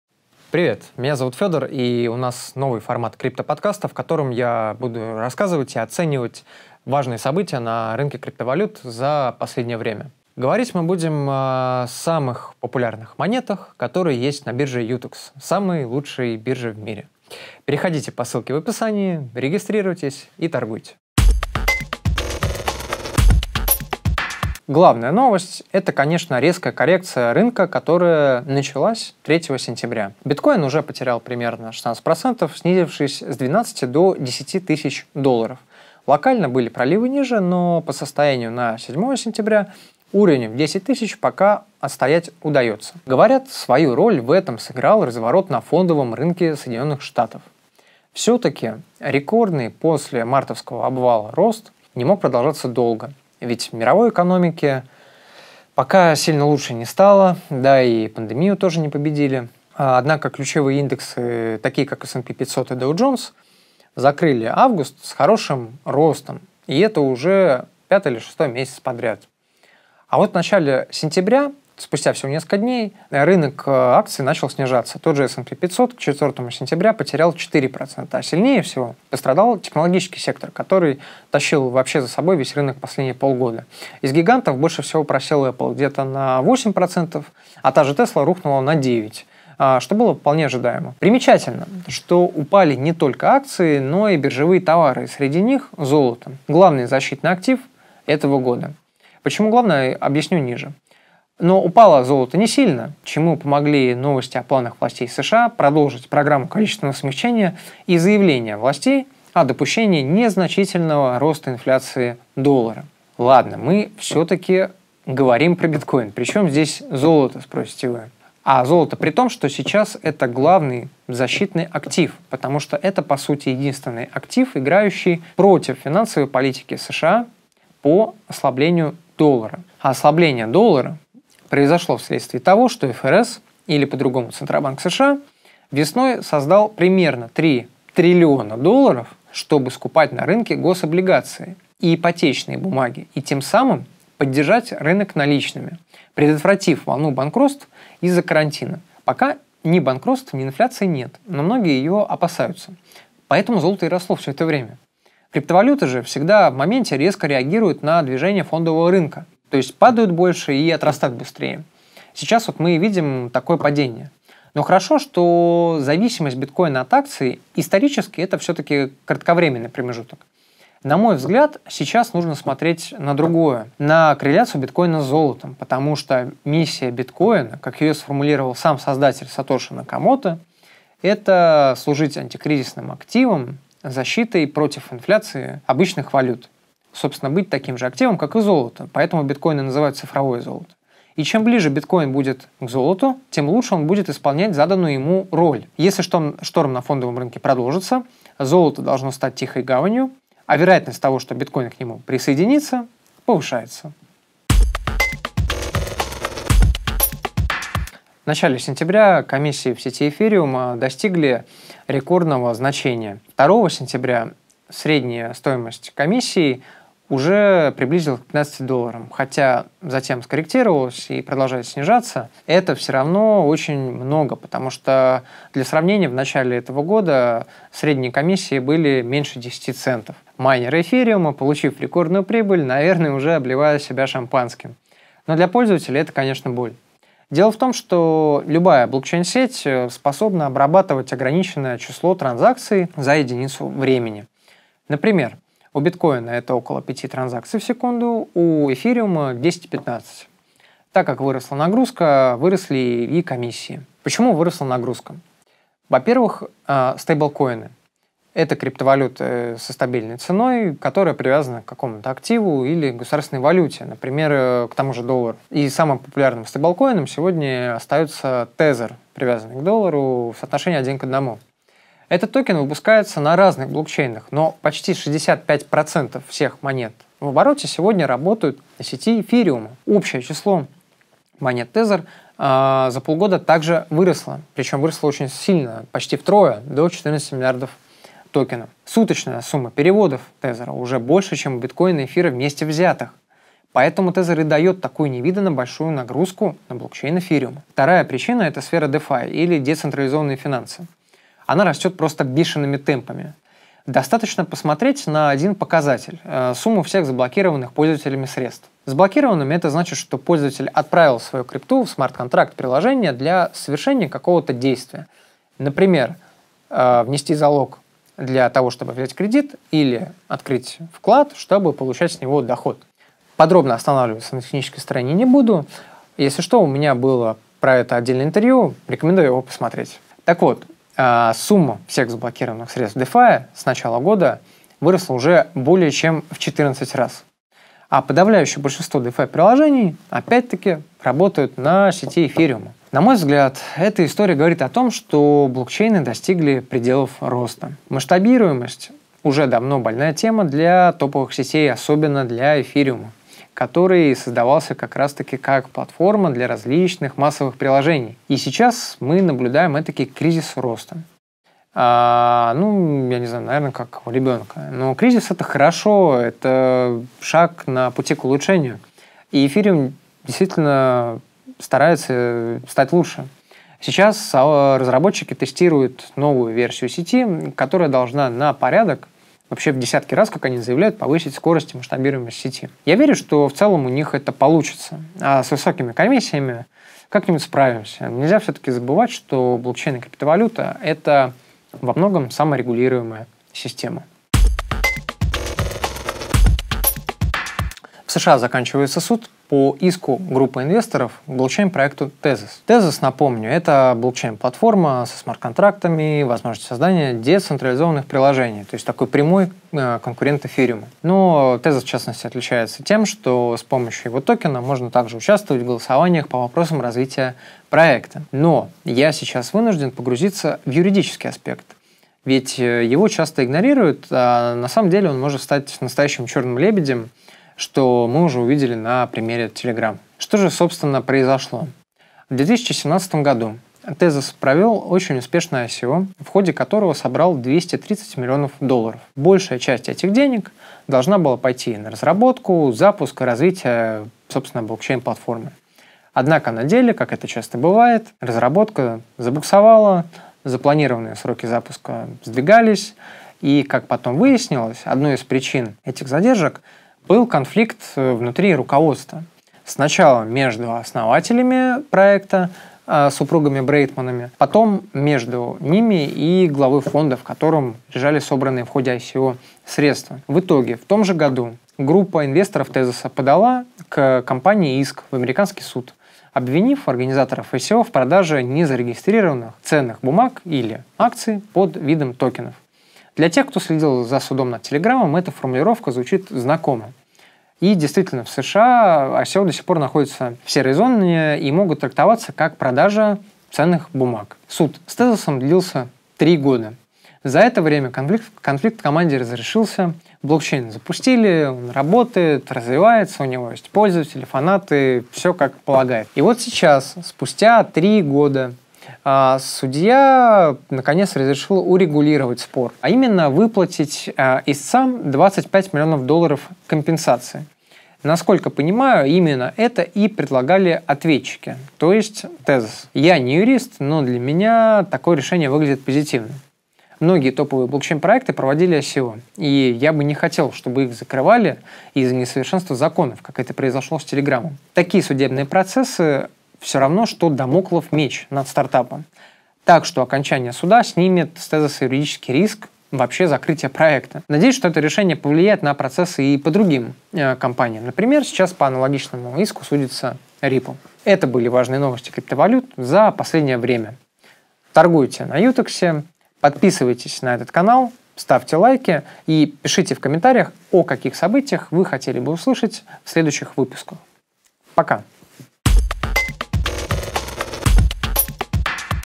Биткоин сцепился с золотом, Ethereum тормозит, Tezos выиграл суд | Крипто подкаст #11
Почему крипторынок упал и что с ним будет дальше, каково скрытое значение недавней перегрузки сети Ethereum, и откуда у держателей Tezos повод вздохнуть свободнее.

00:00 Вступление
00:24 Коррекция рынка криптовалют.
03:42 Комиссия в сети Ethereum
06:52 Tezos выиграл суд